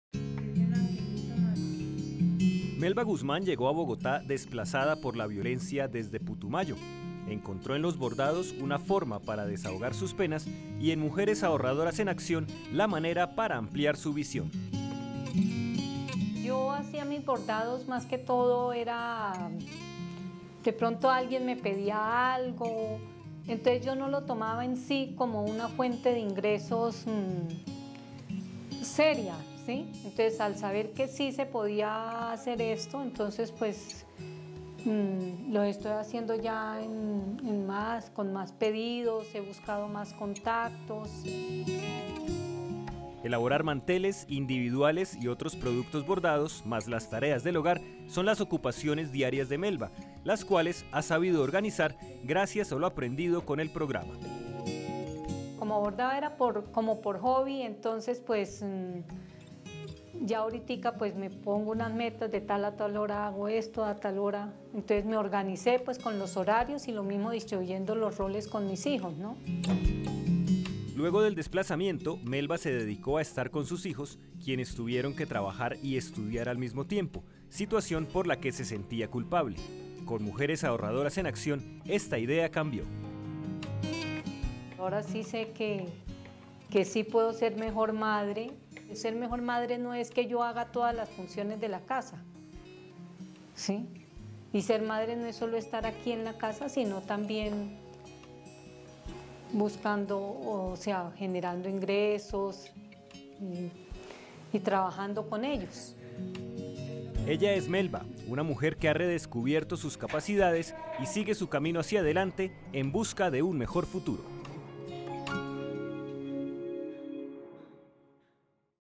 Hilo y aguja
Nury Pineda se graduó el año pasado de Mujeres Ahorradoras en Acción... En el barrio La Paz, en Bogotá, tiene su pequeño taller de costura, el cual ha ido fortaleciendo gracias a lo aprendido en las capacitaciones brindadas por el programa.